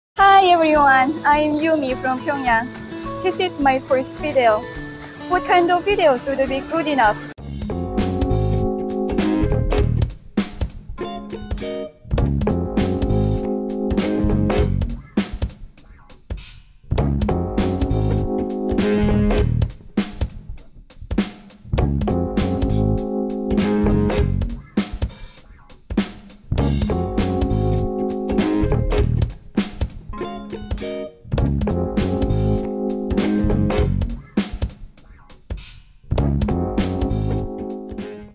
"평양 사는 유미" 요가하고 먹방찍는데 수상하다
'유미'라는 이름의 북한 소녀가 평양에서의 생활을 소개하는 영상을 유튜브에 게시하고 있습니다.